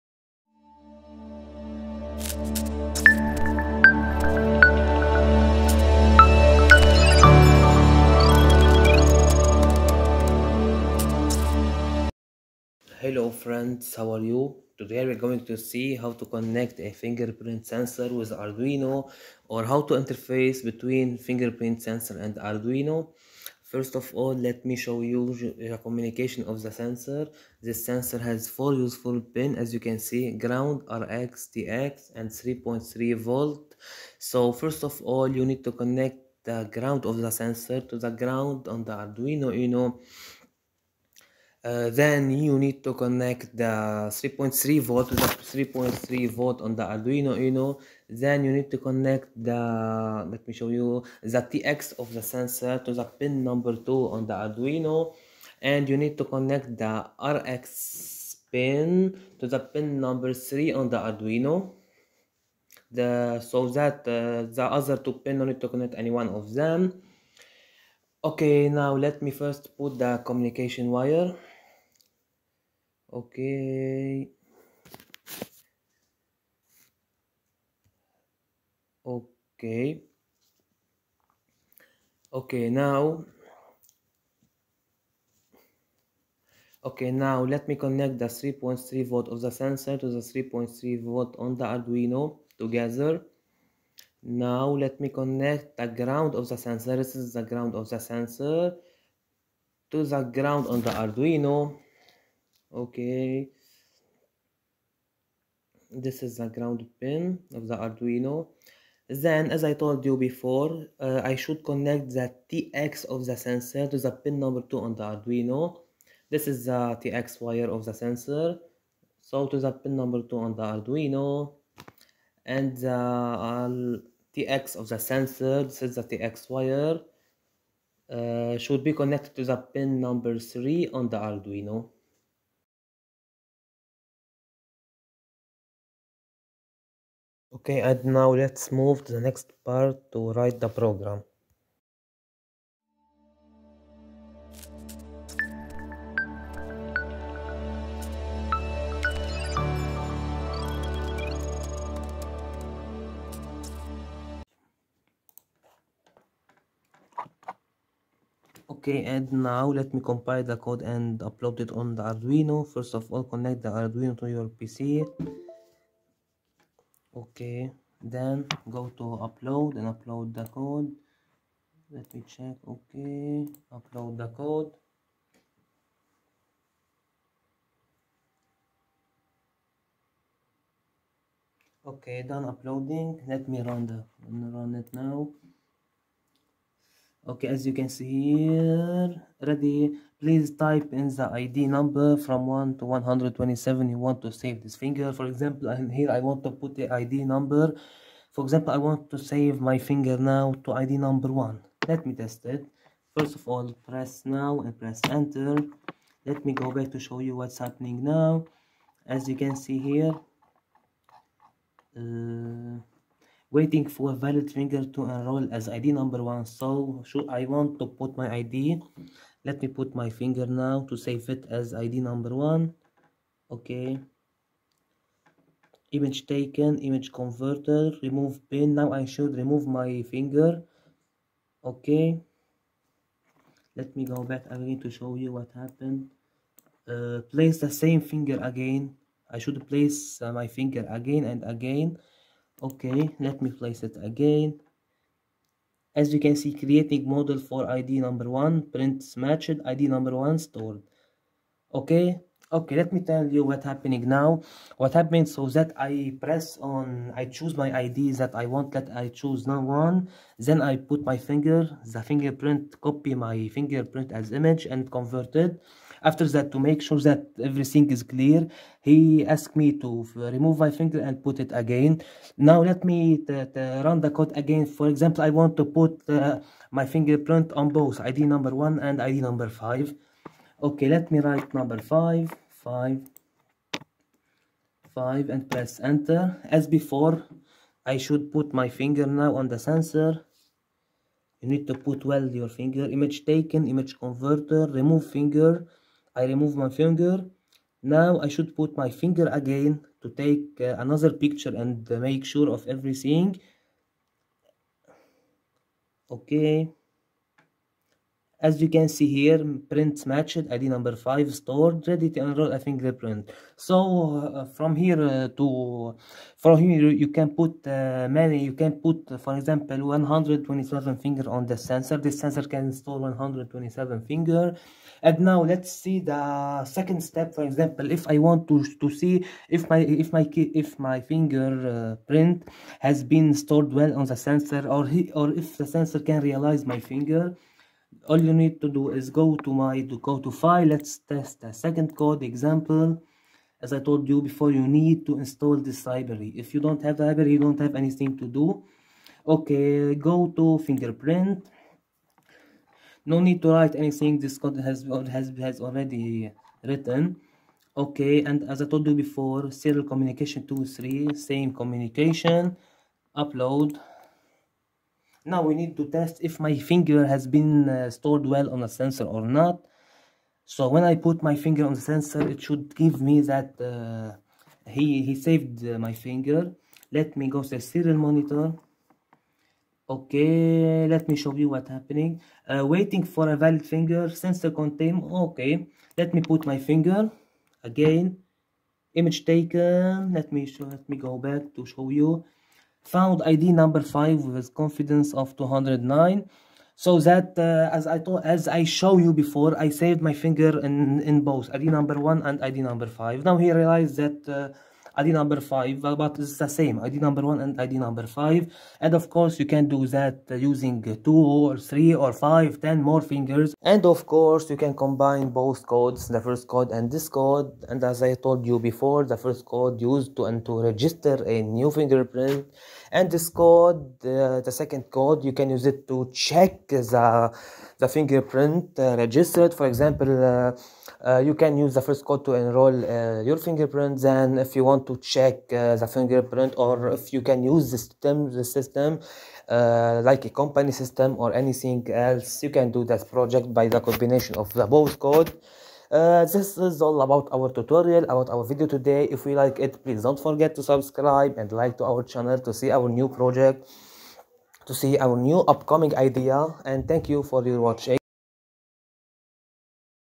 How to connect a finger print sensor with Arduino Uno
This video Shows you how to connect or how to interface between a finger print sensor and Arduino.😊
No need to write anything, simply you need to install the library and watch the steps to start working on your project.